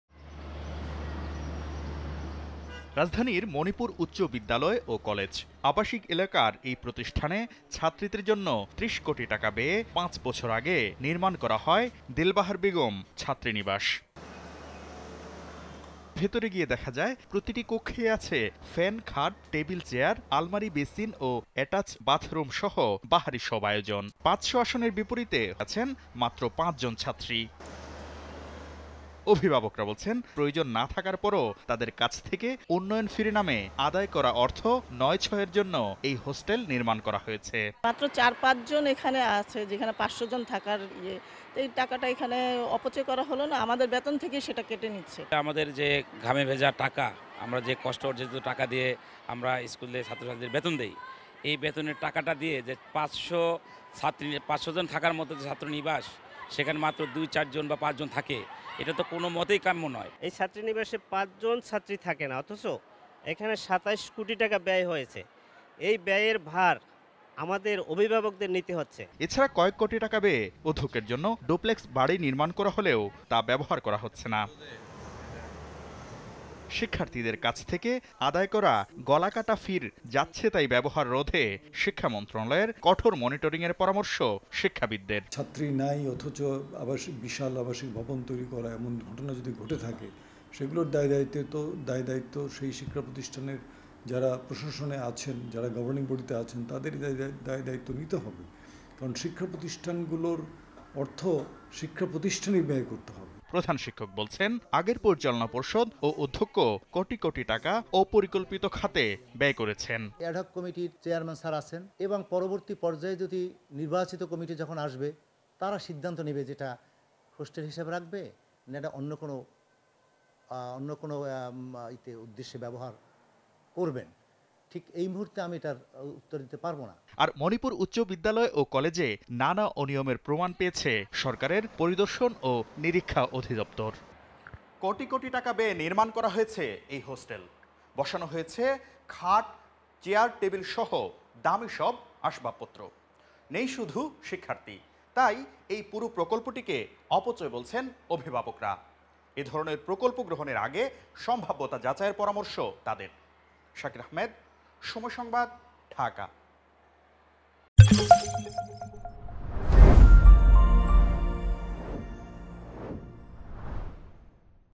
৫০০ জন ছাত্রীর জায়গায় থাকে পাঁচজন | Girls Hostel | Somoy TV
৫০০ জন ছাত্রীর জায়গায় থাকে পাঁচজন | Girls Hostel | Somoy TV

৫শ ছাত্রীর হোস্টেলে থাকেন মাত্র পাঁচজন। মিরপুরের মতো আবাসিক এলাকায় প্রয়োজন না থাকার পরও শিক্ষার্থীদের টাকায় মনিপুর উচ্চ বিদ্যালয় ও কলেজে হোস্টেল নির্মাণকে অর্থ নয়-ছয়ের আয়োজন বলছেন অভিভাবকরা। আগের ম্যানেজিং কমিটির ওপর দায় চাপিয়ে প্রধান শিক্ষক বলছেন, হোস্টেলটি অন্য কোন কাজে ব্যবহার করা যায় কি-না তা নিয়ে চিন্তা ভাবনা চলছে।